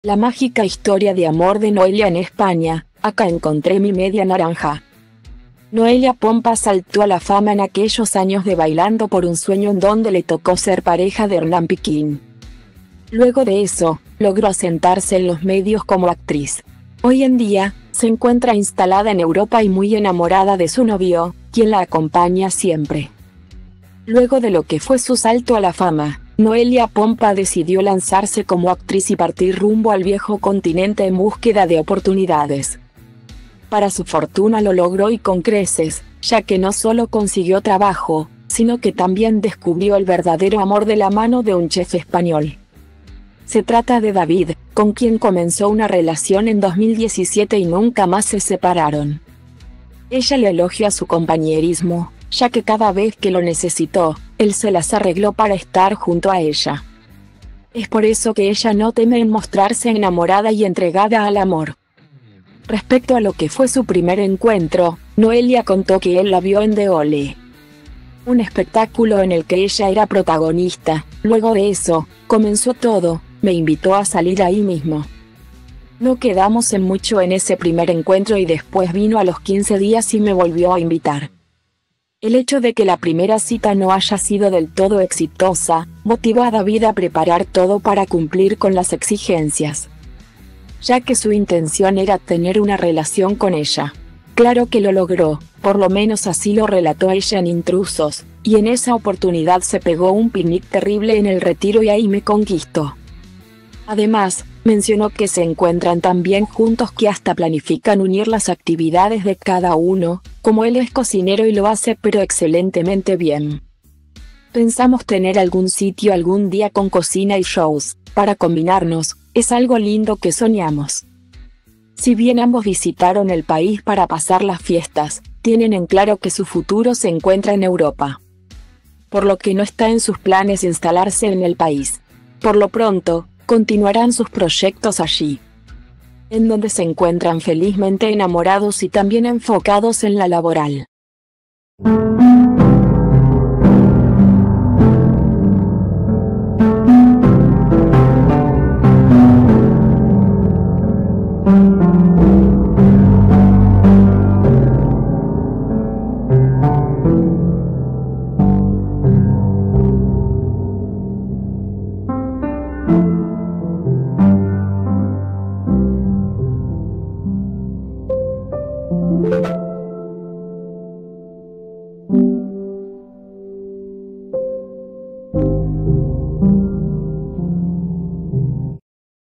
La mágica historia de amor de Noelia en España: “Acá encontré mi media naranja”
Noelia Pompa saltó a la fama en aquellos años de Bailando por un sueño en donde le tocó ser pareja de Hernán Piquín.Luego de eso, logró asentarse en los medios como actriz. Hoy en día, se encuentra instalada en Europa y muy enamorada de su novio, quien la acompaña siempre. Luego de lo que fue su salto a la fama, Noelia Pompa decidió lanzarse como actriz y partir rumbo al viejo continente en búsqueda de oportunidades.